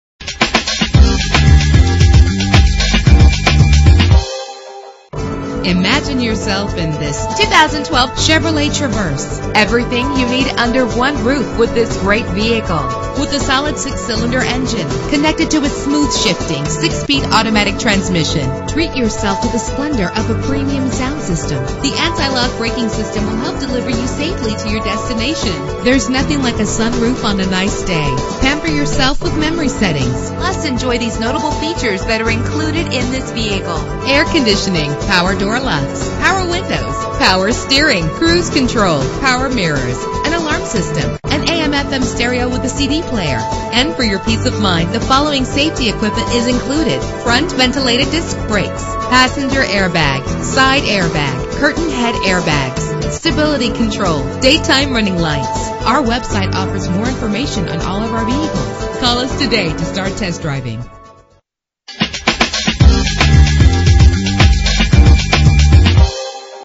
2012 Chevrolet Traverse - Cincinnati OH
Year: 2012
Make: Chevrolet
Model: Traverse
Trim: LTZ
Engine: 3.6 liter 6 cylinder 24 valve
Transmission: 6-Speed Automatic
Color: Blue
Mileage: 1
Address:
9673 Kings Automall Rd